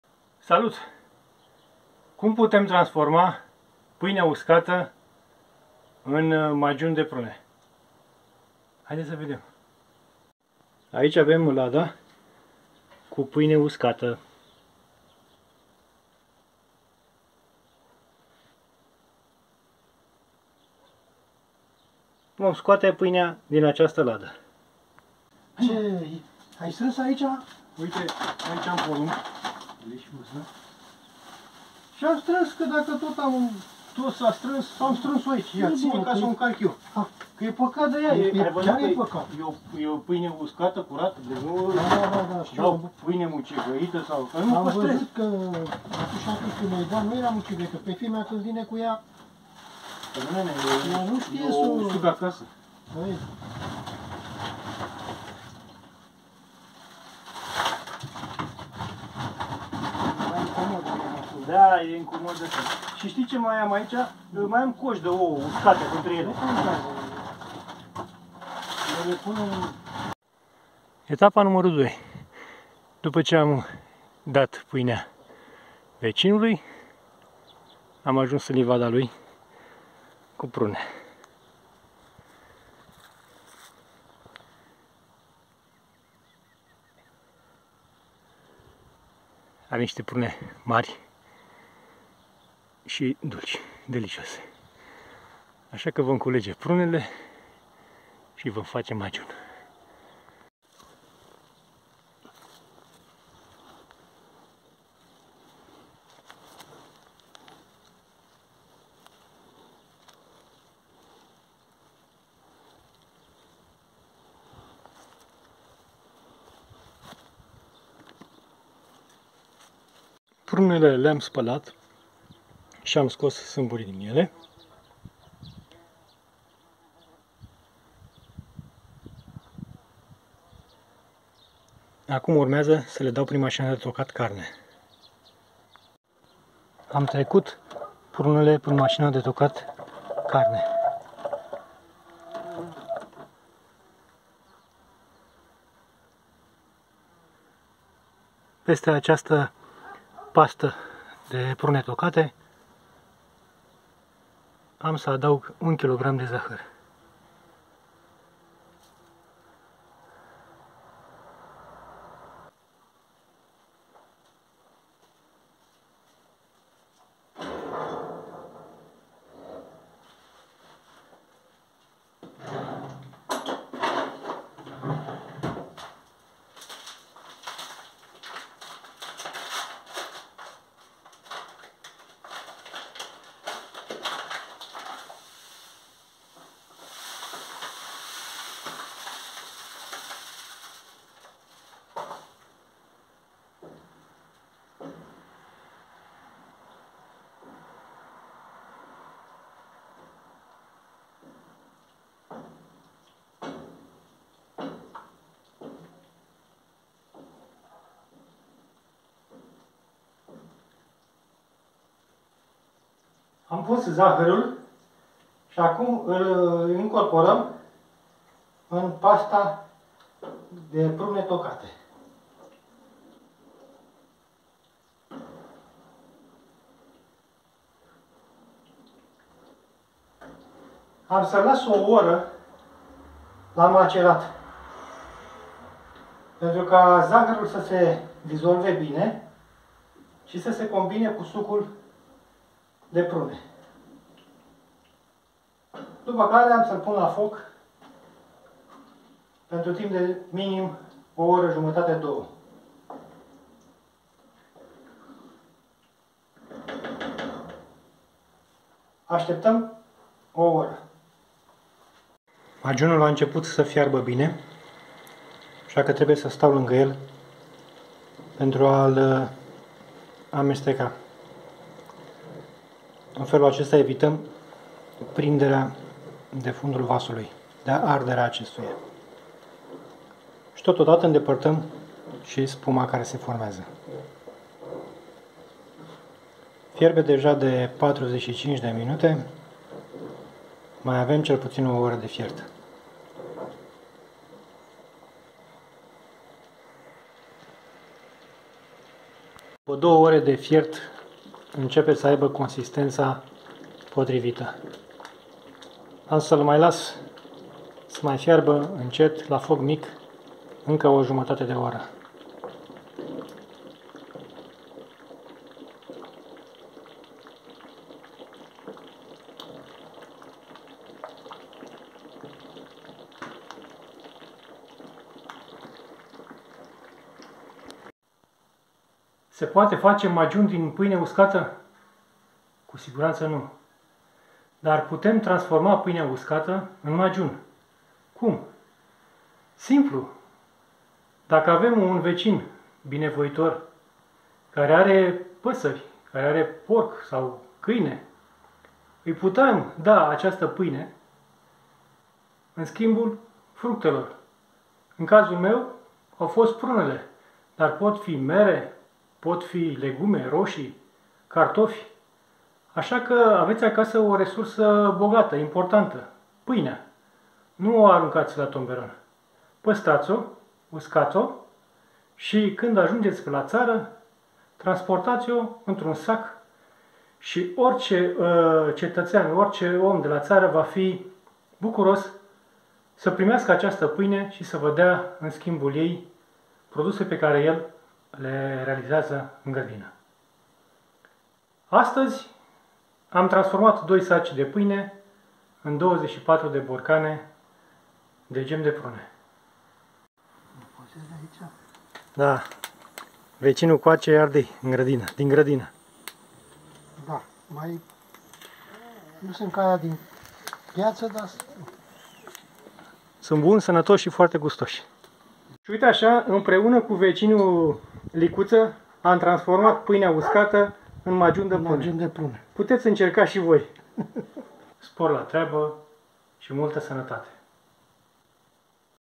Gem din prune luate la schimb!
Painea uscata din gospodarie este o resursa importanta pe care o puteti da in schimbul fructelor.